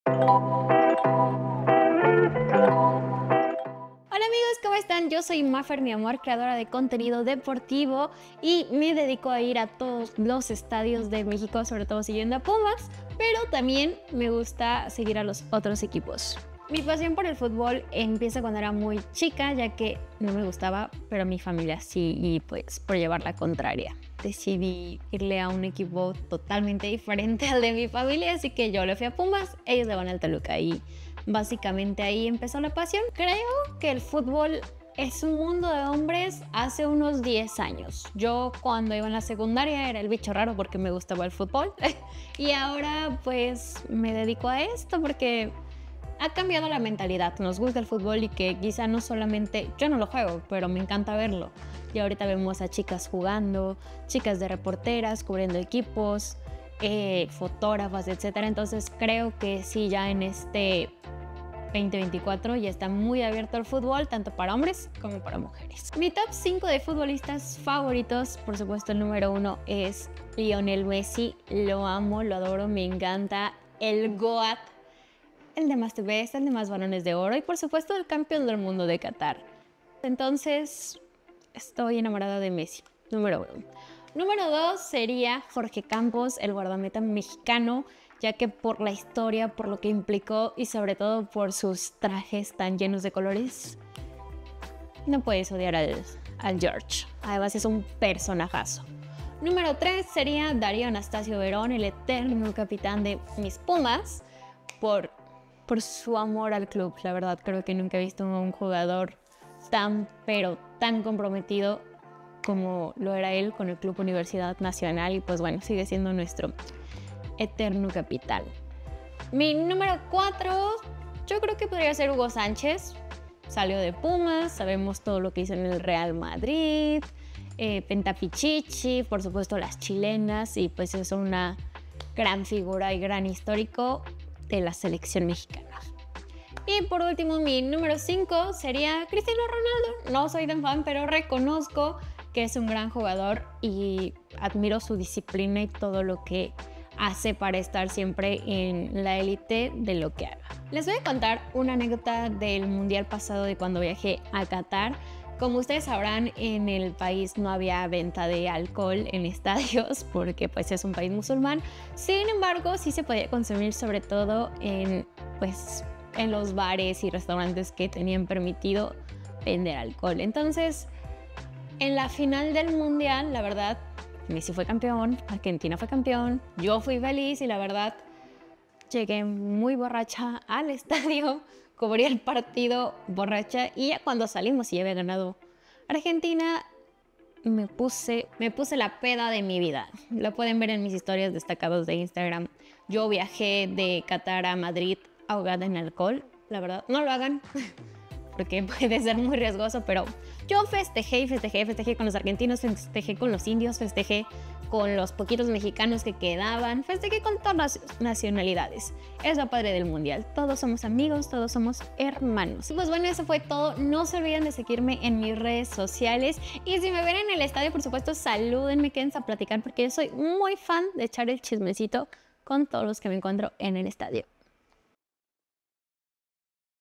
Mafer Miamor, la fan número uno de los Pumas
A Mafer Miamor la puedes encontrar en el Estadio Universitario de los Pumas. Es una de las hinchas más entusiastas de los Pumas. Pero además, es dueña de una sonrisa y unos ojos inconfundibles. La pasión con que la que celebra los goles se contagia.